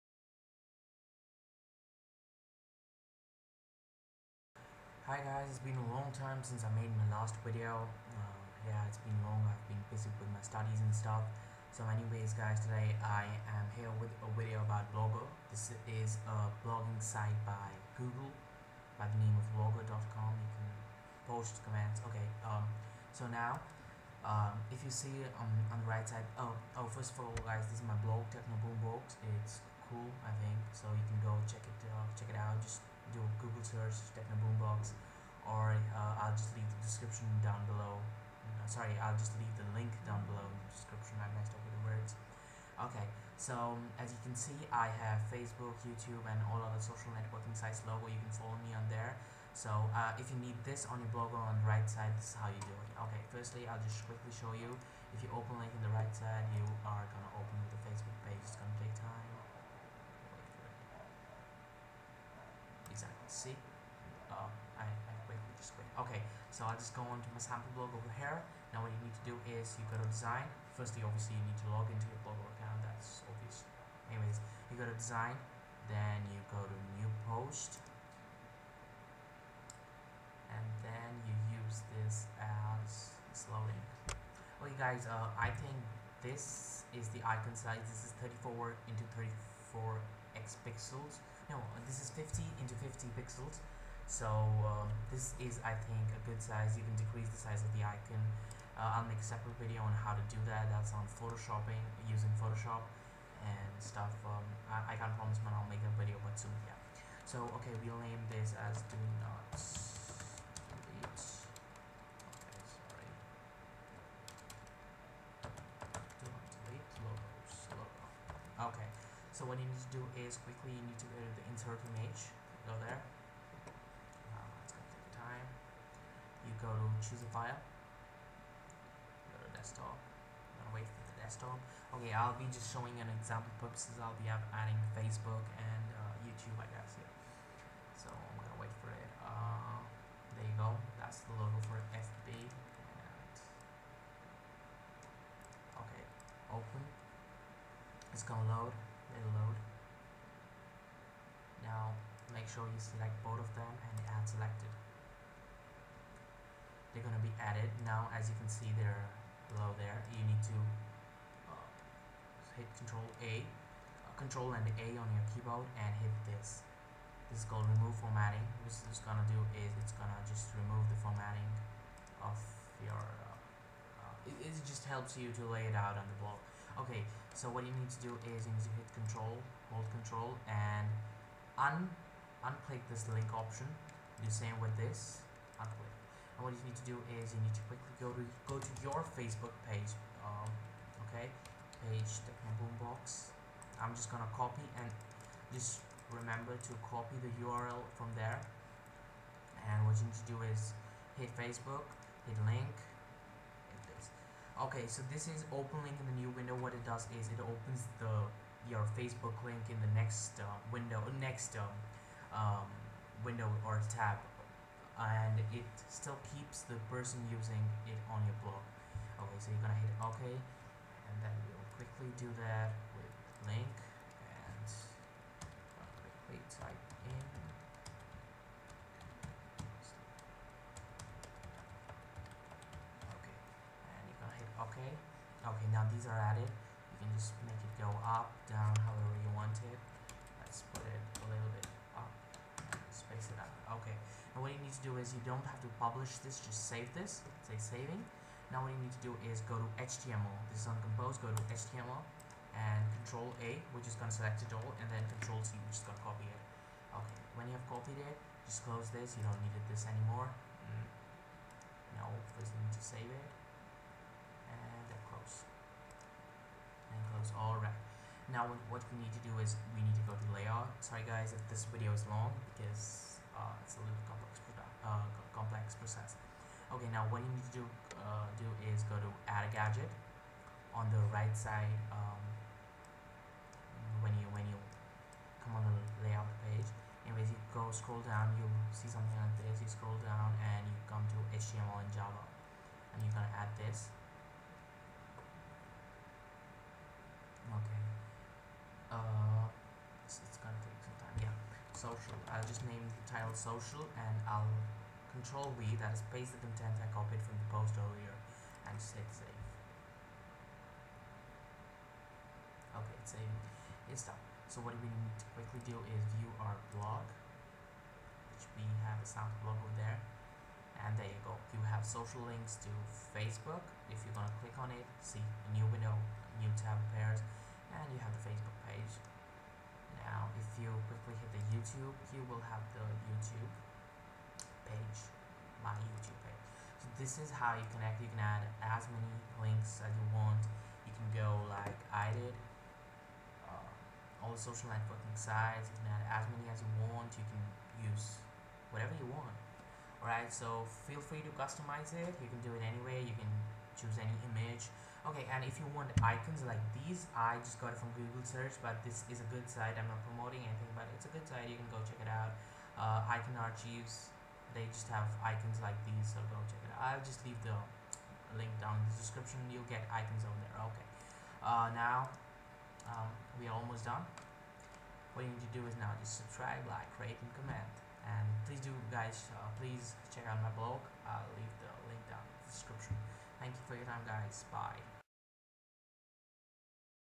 How to add social networking buttons to blogger sidebar
How to add social media buttons to blogger sidebar.
Simple,quick and easy.